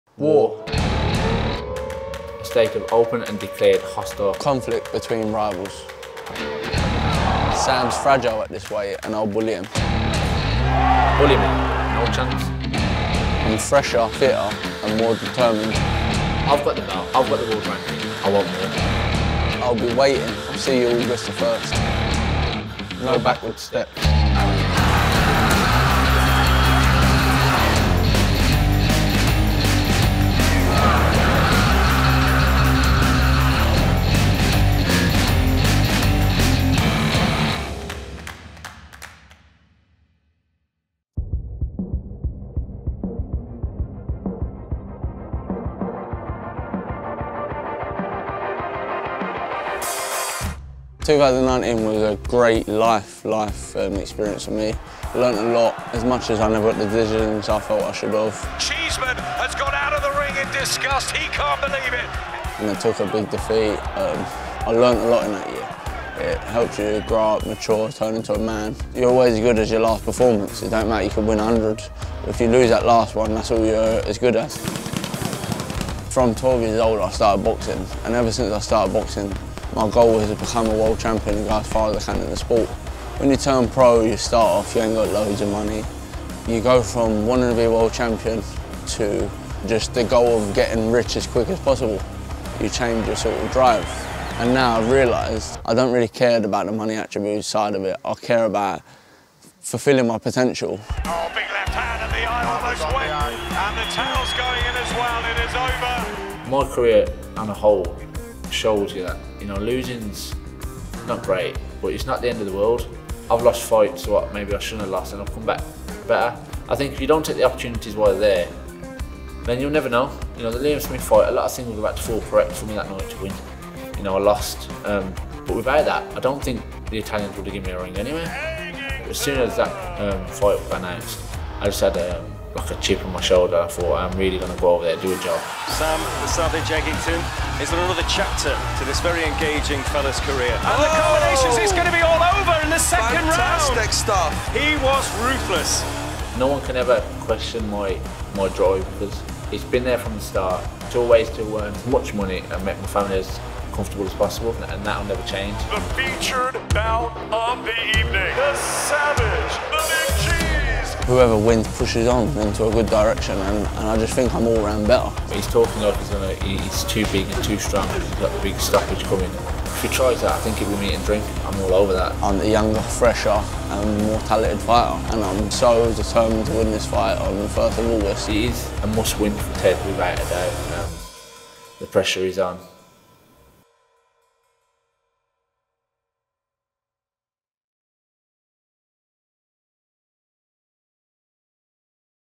Fight Camp Promo: Sam Eggington vs Ted Cheeseman
This has all the makings for all out war! Our Week 1 main event on August 1st sees Sam Eggington and Ted Cheeseman put it all on the line at Fight Camp